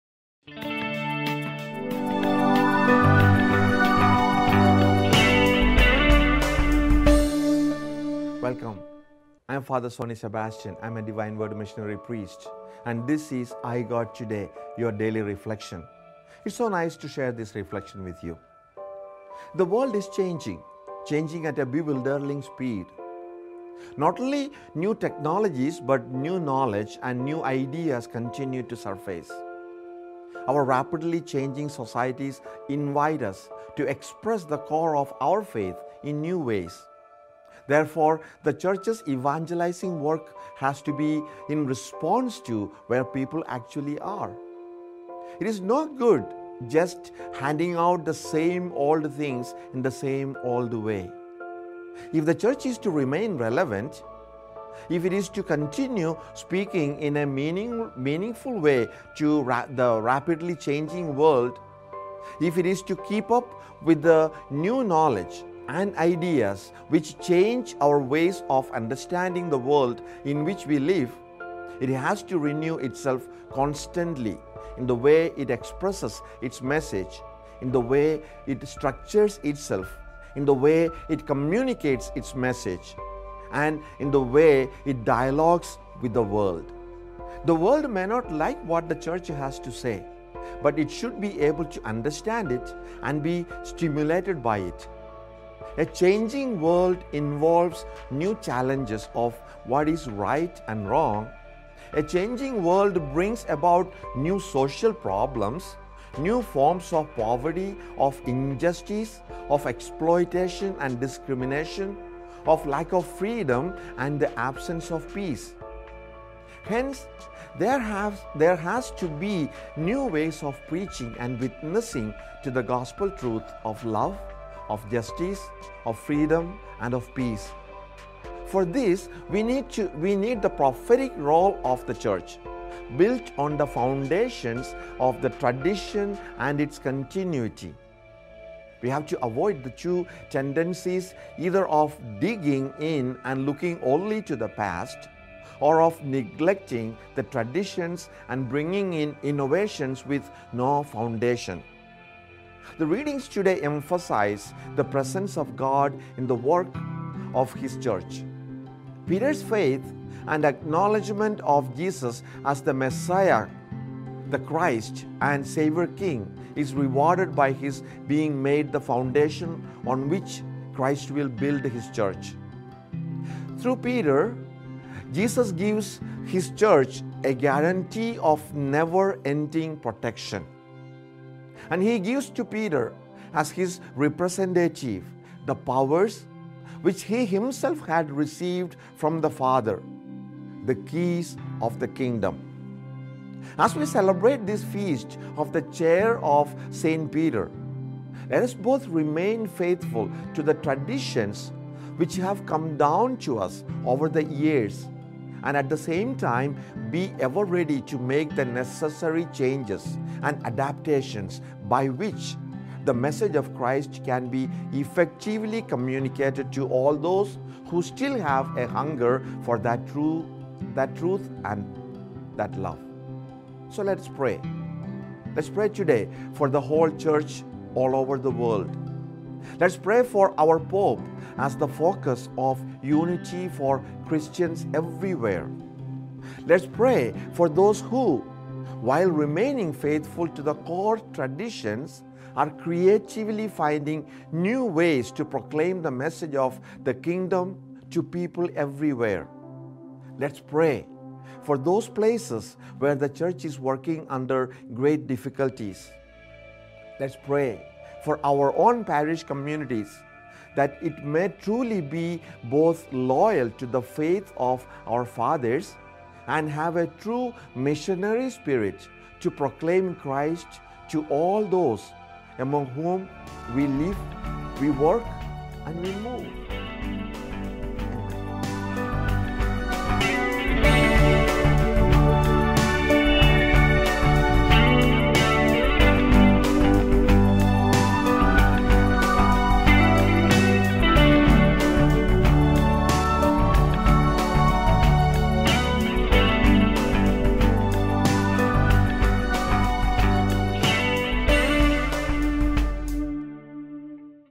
iGod Today with Fr. Soney Sebastian, SVD; Today's topic: In this changing world
Let us pray today for the whole Church all over the world; let us pray for our pope as the focus of unity for Christians everywhere; let us pray for those who, while remaining faithful to the core traditions, are creatively finding new ways to proclaim the message of the Kingdom to people everywhere; let us pray for those places where the Church is working under great difficulties; let us pray for our own parish community that it may truly be both loyal to the faith of our fathers and have a true missionary spirit to proclaim Christ to all those among whom we live. Amen.